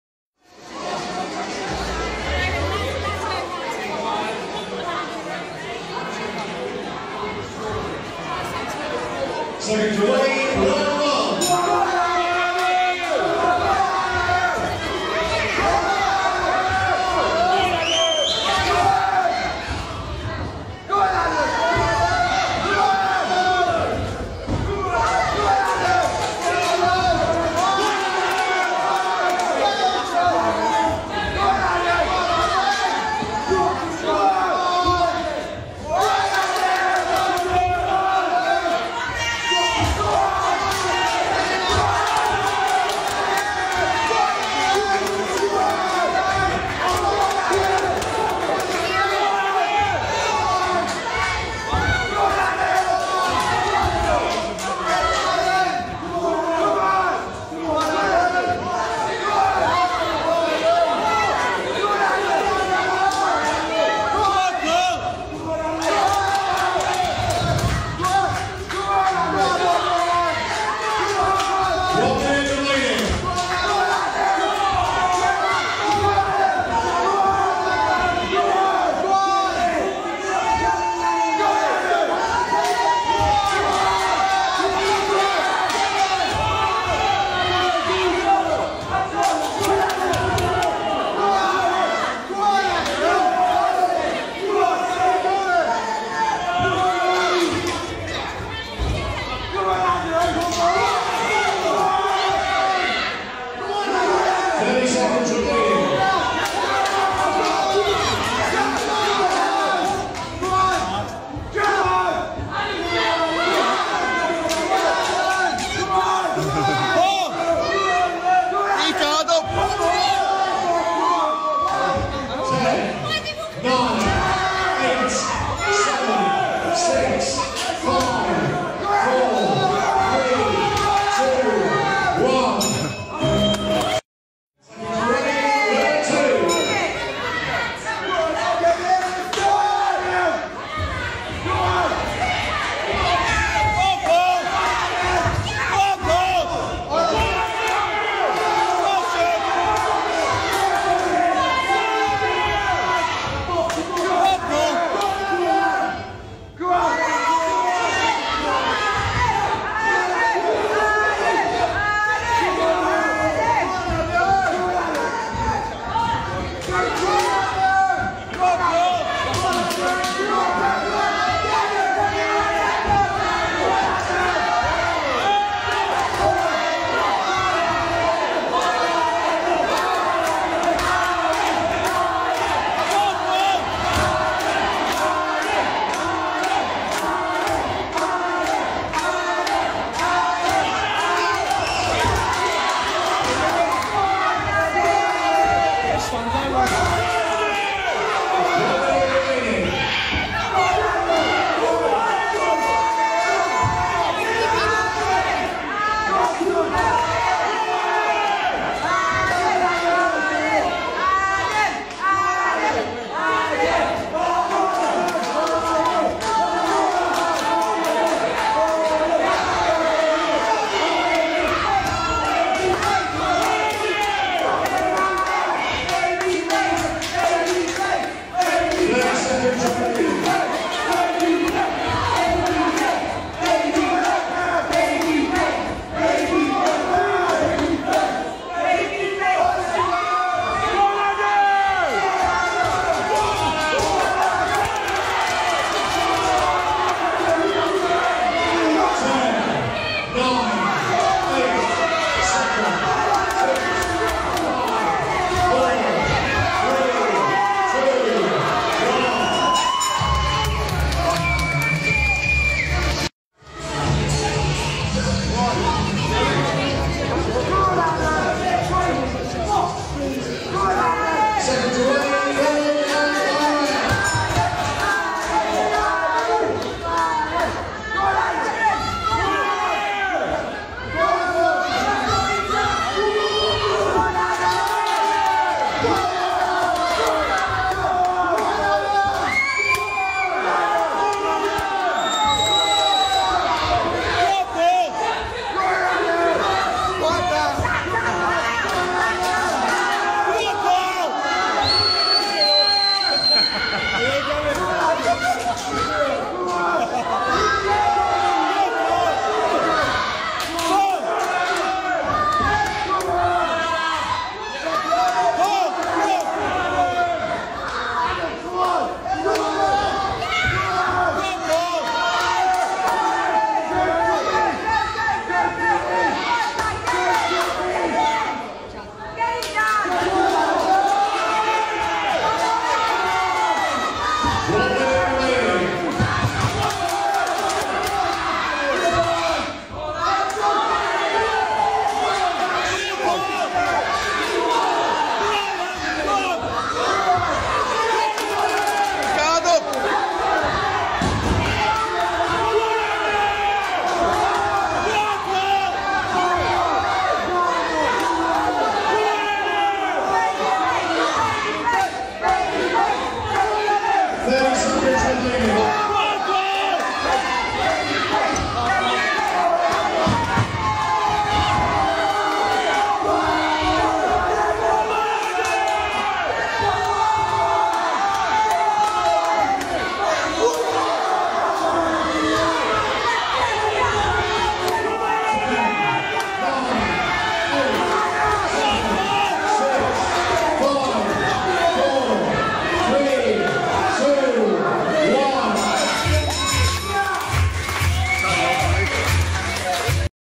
Paul May Vs Adam Dickens Charity Boxing Challenge
Paul May (red) Vs Adam Dickens (blue) Charity Boxing Challenge ( pink collar boxing )
This is my 3rd fight 2 weeks after my last fight. It was an unscheduled fight at The Waterfall In Derby.